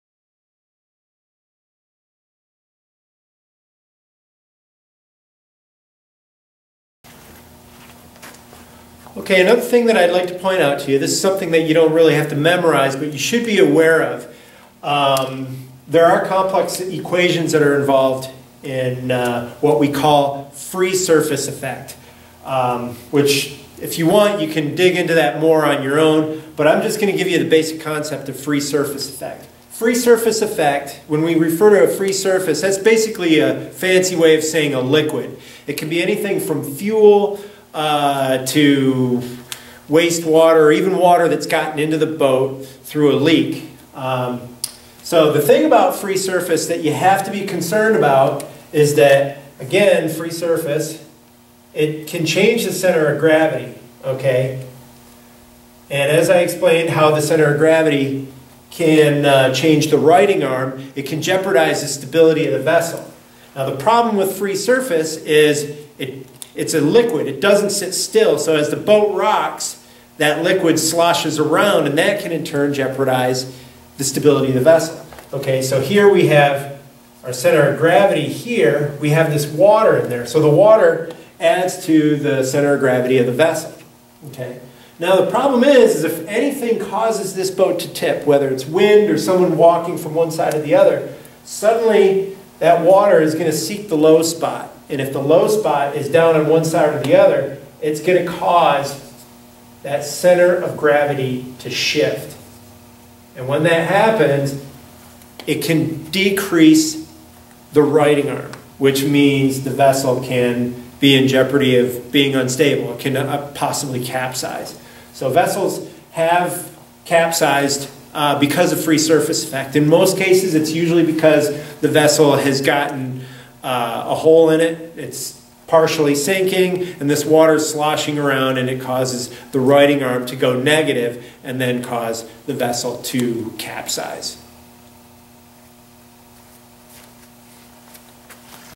Stability Unit, Part 3: Free Surface Effect
Content for Lake Superior State University (LSSU) course on Boat Handling and Navigation. Lectures by Captain Benjamin Hale, provided by Inland Seas Education Association.